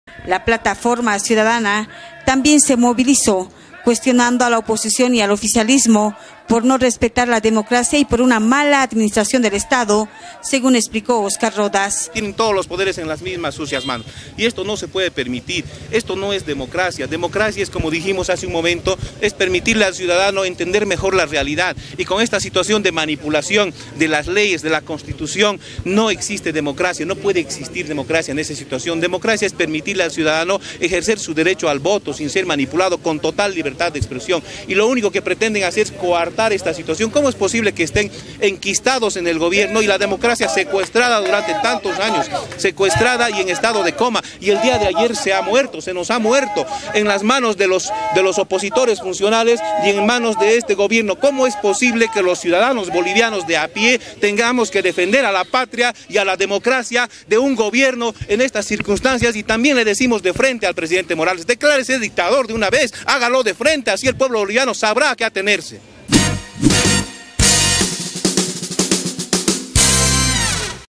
OFICIALISTAS Y OPOSITORES SON CUESTIONADOS POR LOS CIUDADANOS
La plataforma ciudadana se movilizó en contra del fallo de TCP, por no respetar la democracia y una mala administración del Estado.